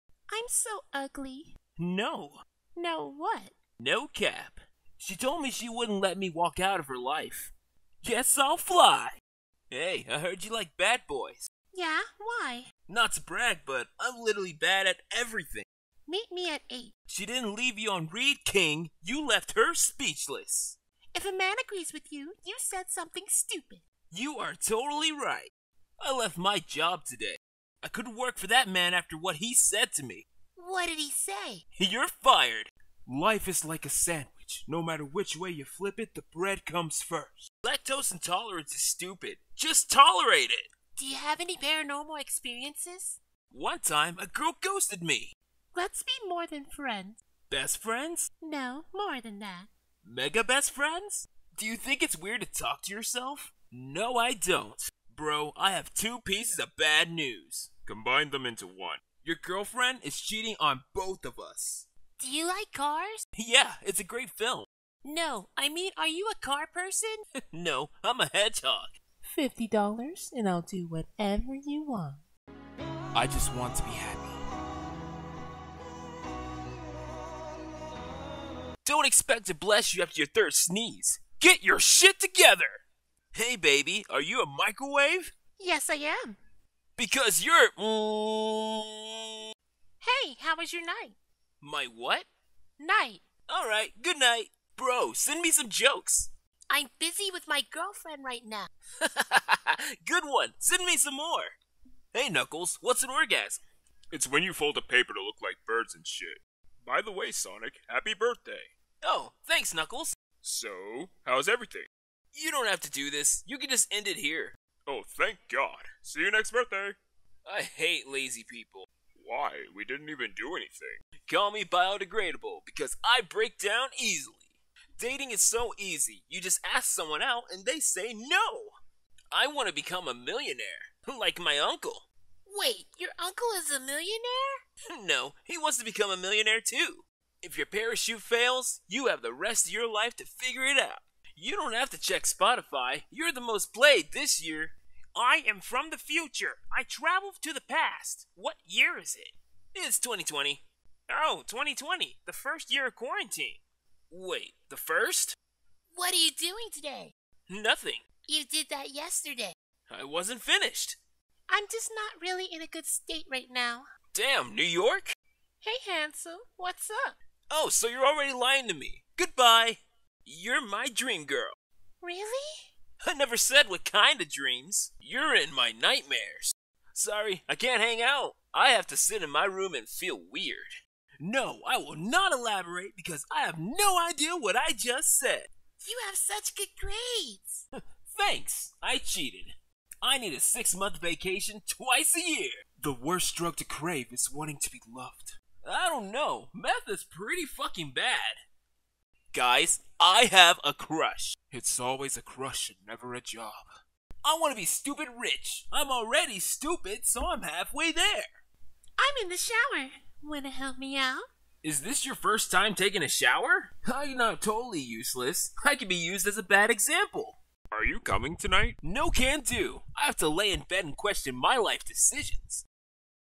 Sonic Meme Dubs - TLtheArtist edition
Took some time to gather my favorite Sonic memes from @EliteSonicFan on Twitter over the last few years and voice em back to back xD thanks for making my days between work great~

All voices by me